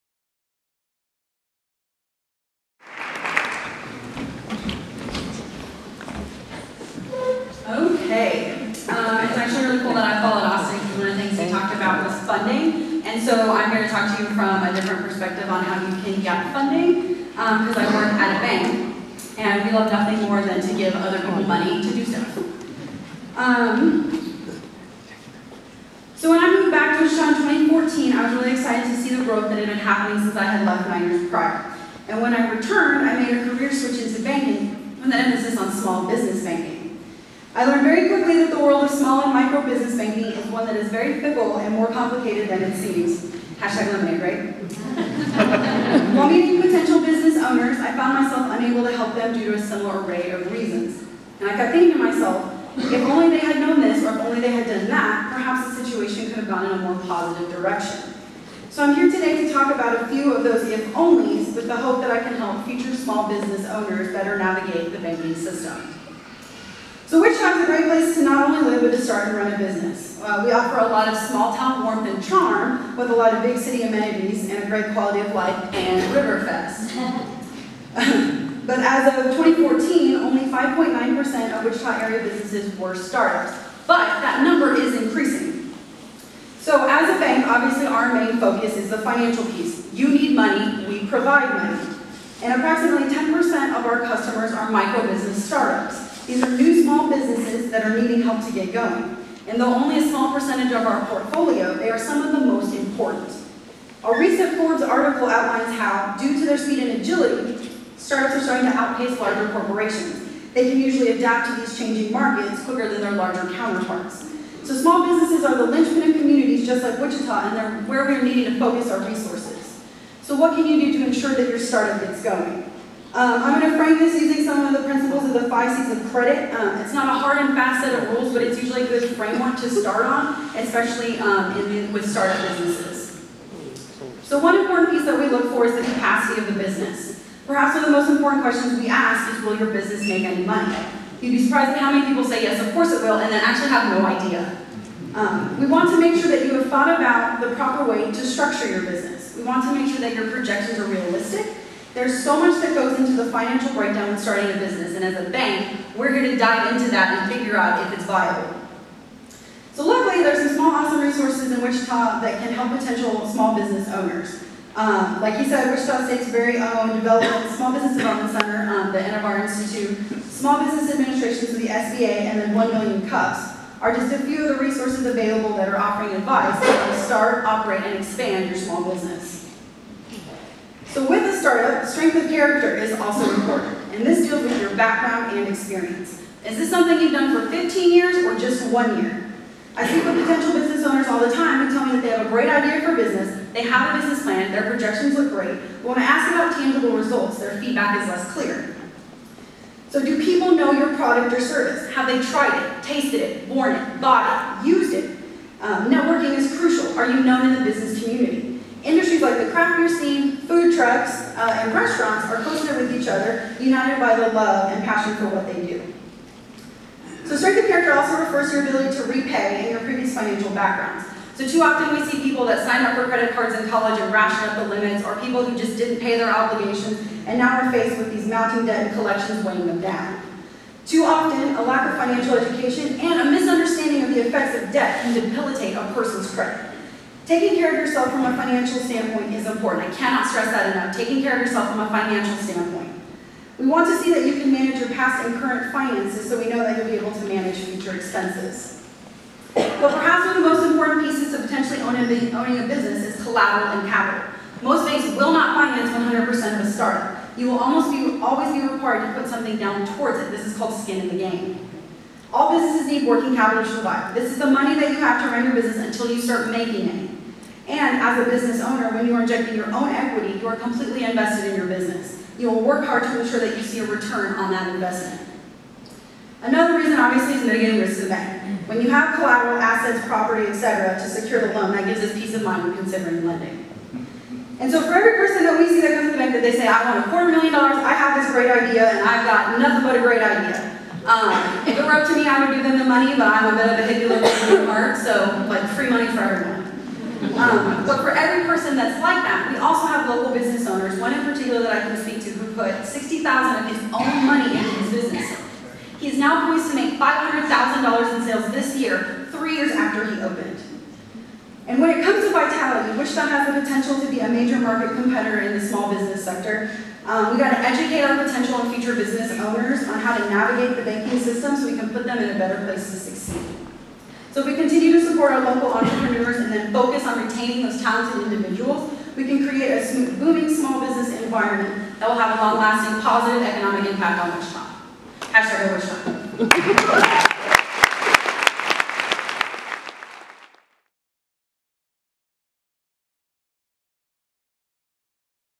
Tristan Neth - The Big Business of Small Business - Opportunity in the ICT
Tristan is a born and raised Wichitan whose pride in her hometown is evident within minutes of talking to her. She’ll gladly tell stories of the fun she had growing up in Wichita; playing down by the river with her brothers, eating at Sub ‘N’ Stuff and Spaghetti Jacks, playing on the rocketship in Riverside park (Bring Back the Rocket!) and attending every year of RiverFest since her birth (and has the buttons to prove it!). After a 9 year hiatus living away during college and after, she is enjoying living back in Wichita with her husband and showing him all that this great city has to offer.

Favorite Thing About Wichita:
“My favorite thing about Wichita is it's potential. It offers all of the perks of a small town melded together with the possibilities of a larger city. Great things are happening here and will continue to happen here - case in point, the burgeoning craft beer scene that is putting us on the map - who doesn't love that?”

 tristan.neth
 tristan-neth

Wichitalks.com for more details.